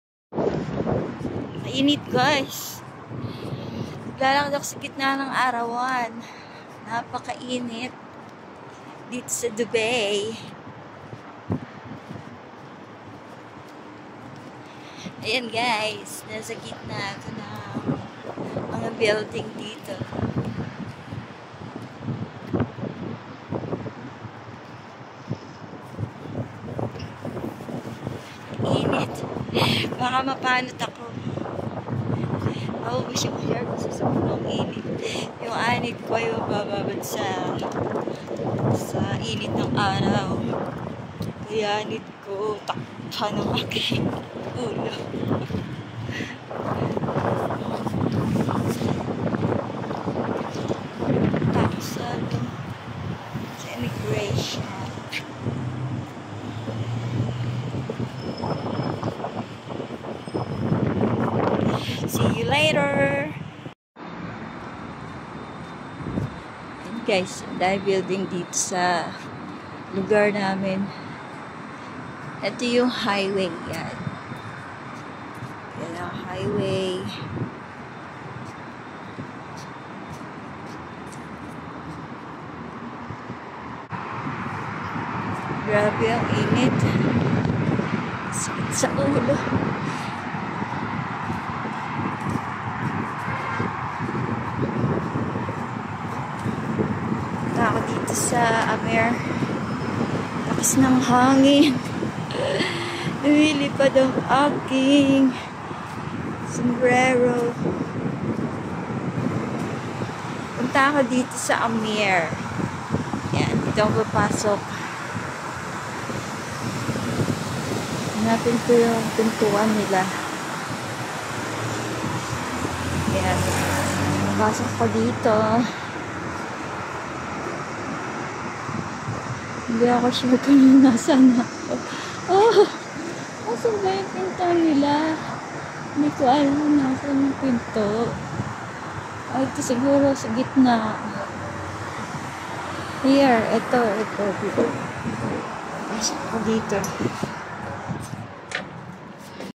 Al NAHDA DUBAI || Shanee Menor Vlogs || OFW Family in Dubai
I walked at the opposite side of our house 🏡 I was going to Amer to pay our bills for our visa. We pay for the hold of our visa so we can process my husband Visa